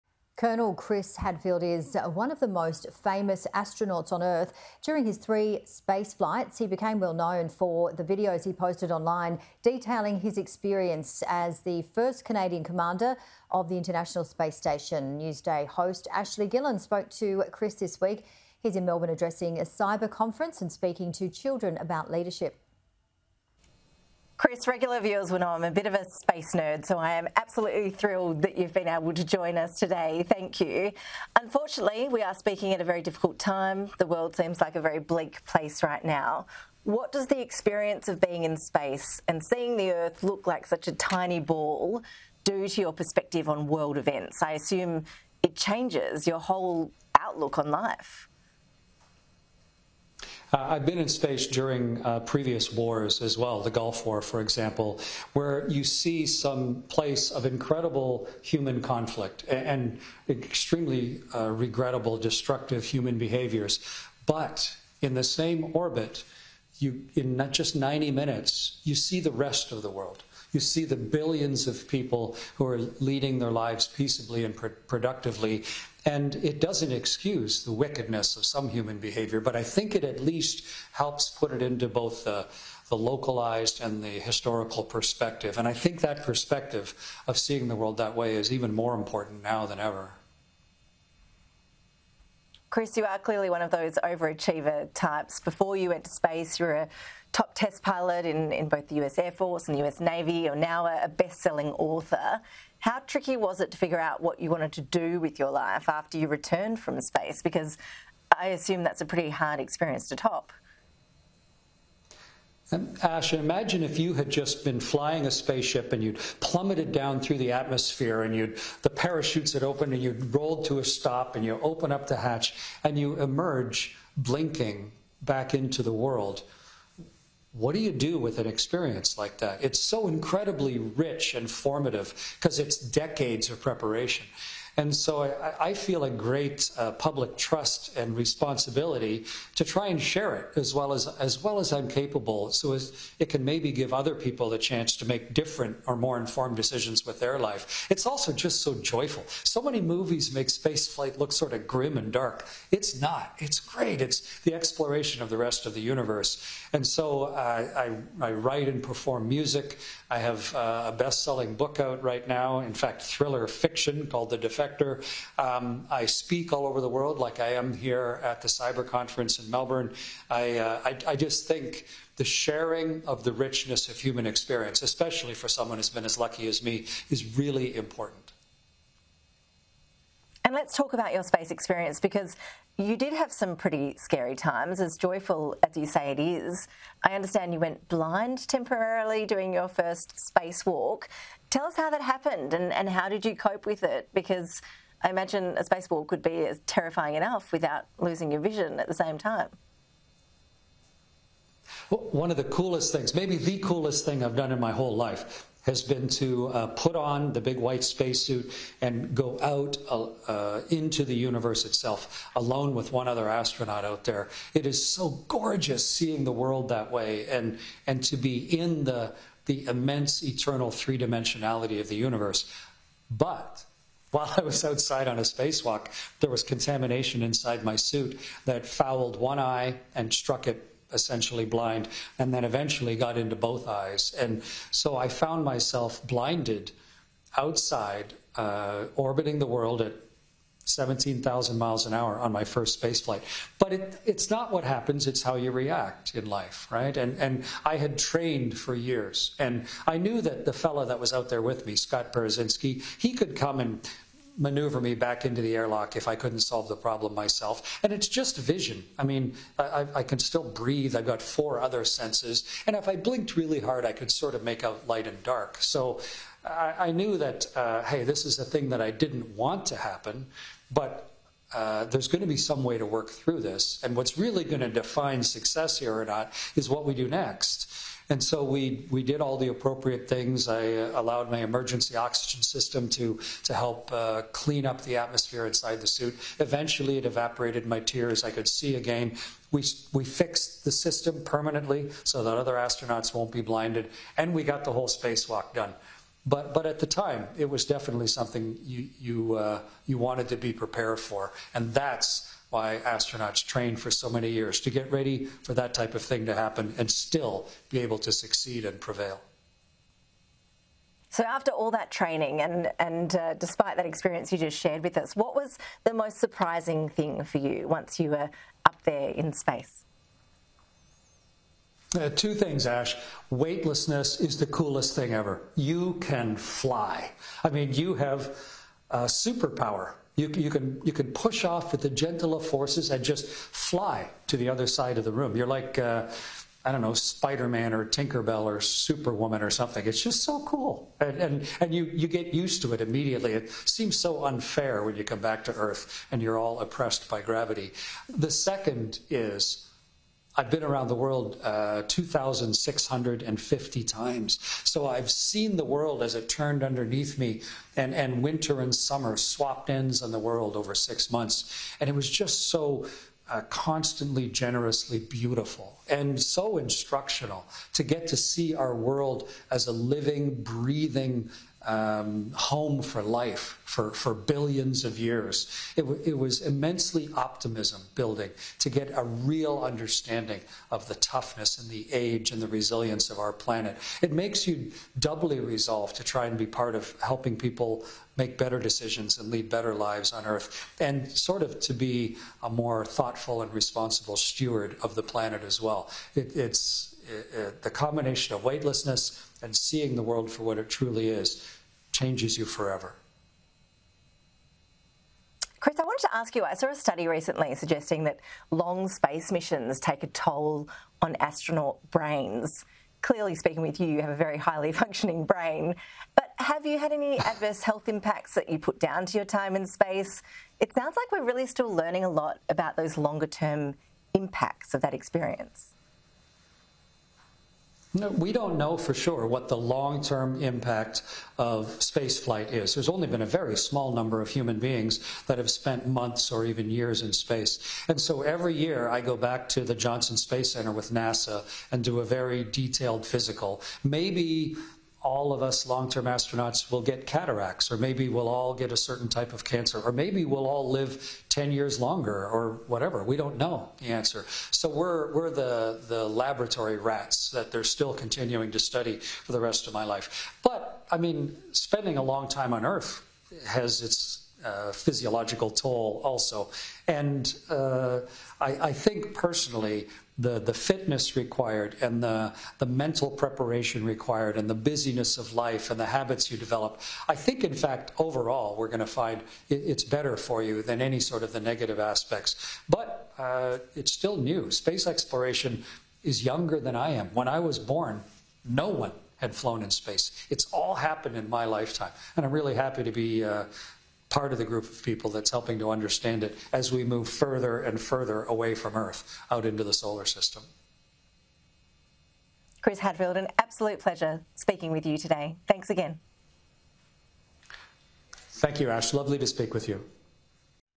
Retired astronaut opens up about his time on the International Space Station
Retired astronaut Chris Hadfield is one of the most famous astronauts on Earth.

During his three spaceflights, he became well-known for the videos he posted online.

The videos detailed his experience as the first Canadian commander of the International Space Station.

Mr Hadfield told Sky News Australia he feels a great “responsibility” to share his experiences of his time in space.

“It can maybe give other people a chance to make different or more informed decisions with their life,” he said.